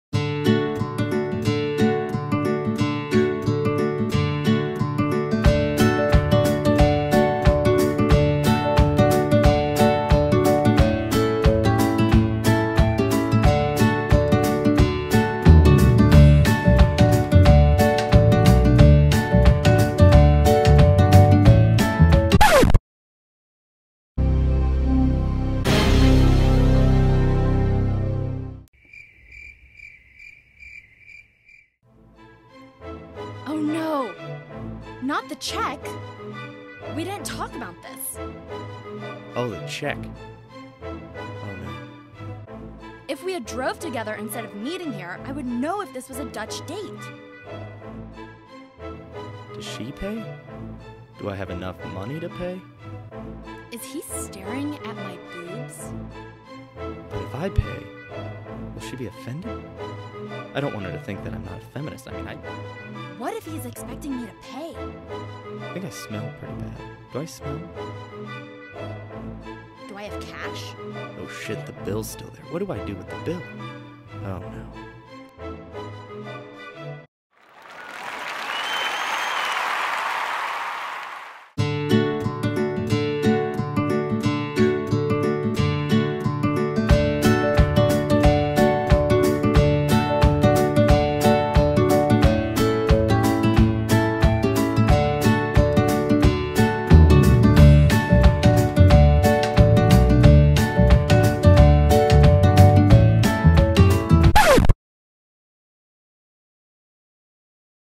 The First Date!
Had fun making this video with my boo bear for an assignment. Not the best editor but I'm working on it!